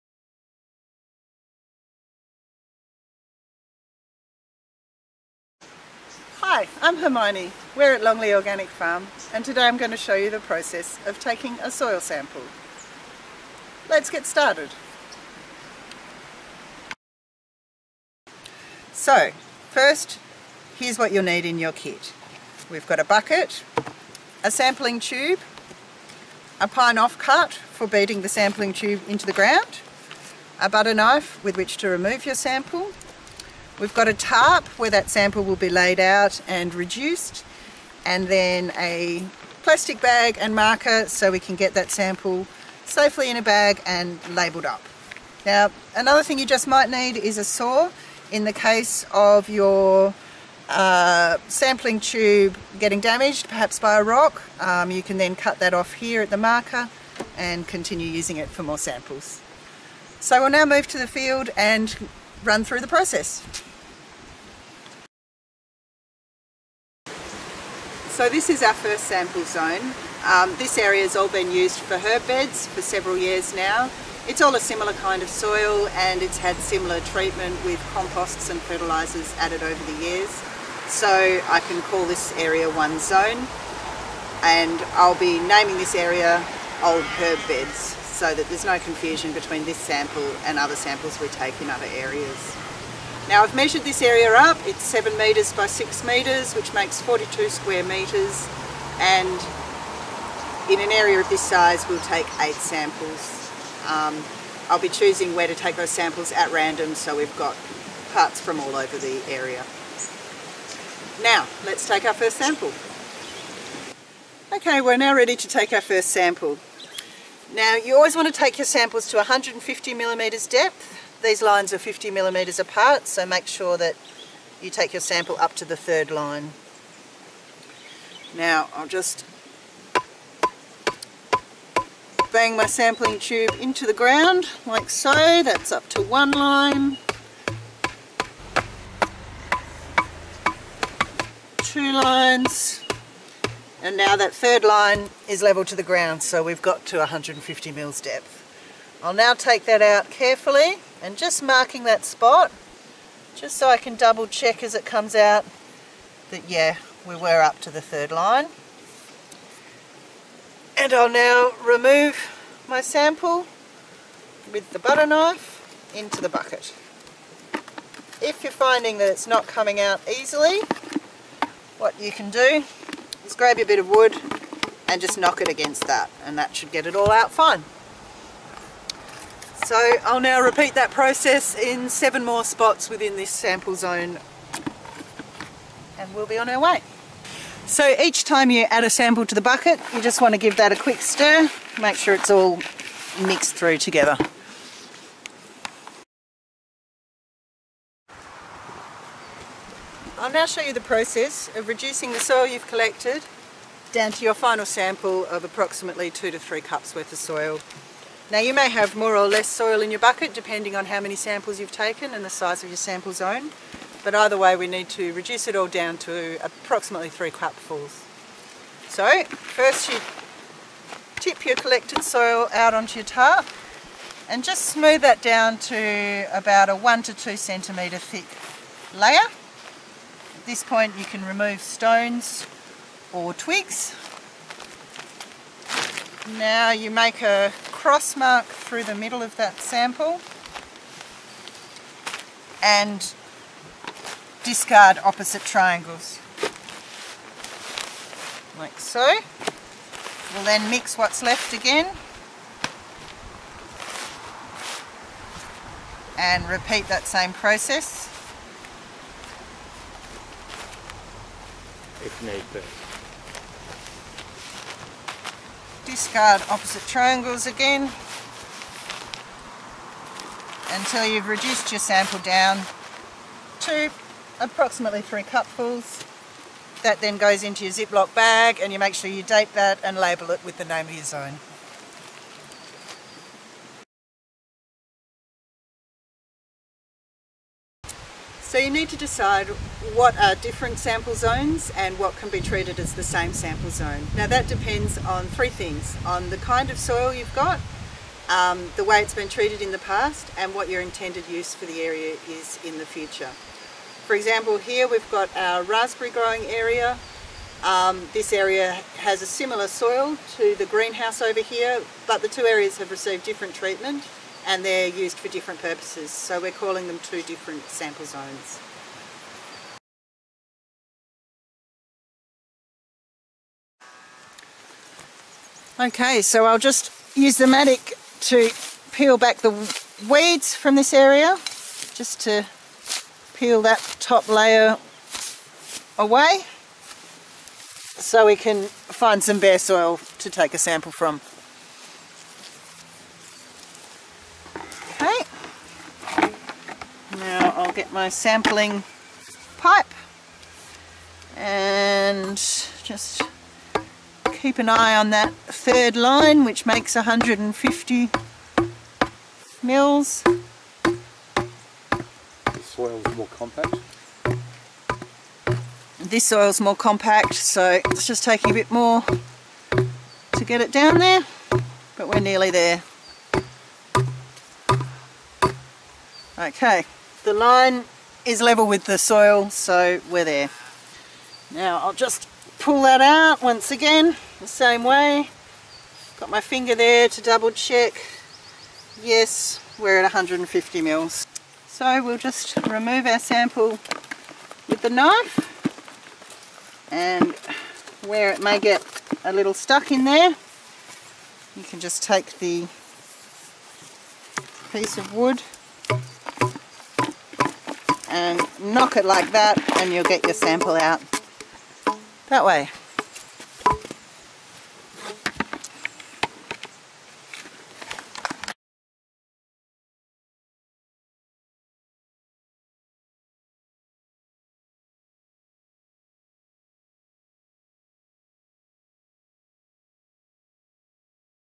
Soil sampling, Longley Organic Farm - Soil Dr Service
Hermione demonstrates the customer DIY sampling kit for our Soil Doctor Service.